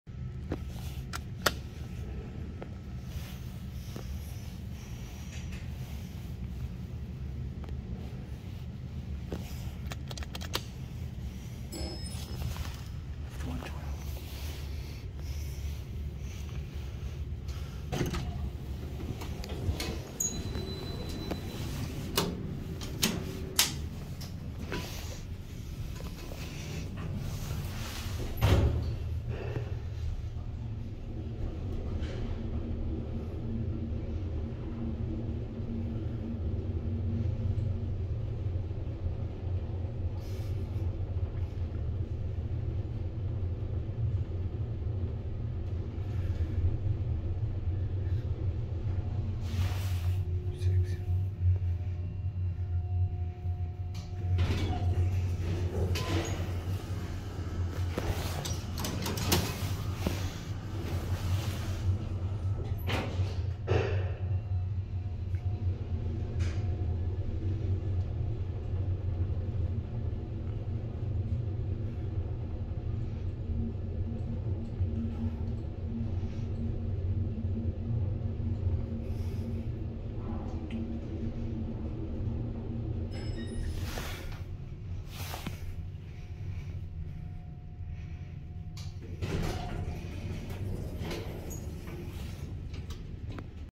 30 Dec 2022: 1965/83 Montgomery Traction Elevator 112 | UW School of Medicine (J-Wing) | Seattle WA
I managed to turn the fan off!!!

Installed 1965

Extended 1983